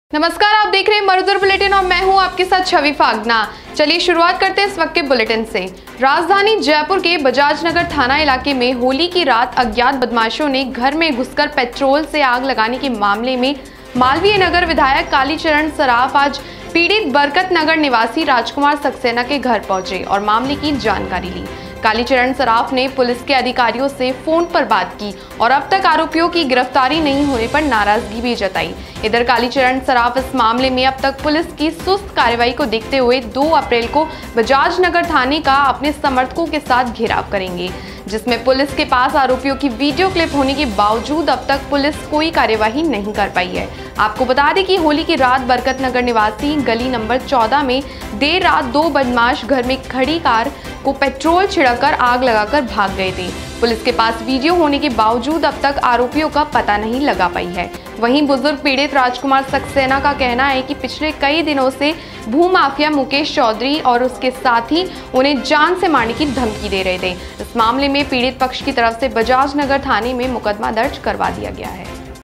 Police के पास Video Clip होने के बावजूद अभी तक नही हुई कोई गिरफ्तारी || Jaipur Latest News
Police के पास Video Clip होने के बावजूद अभी तक नही हुई कोई गिरफ्तारी, Kali Charan Saraf ने जताई नाराजगी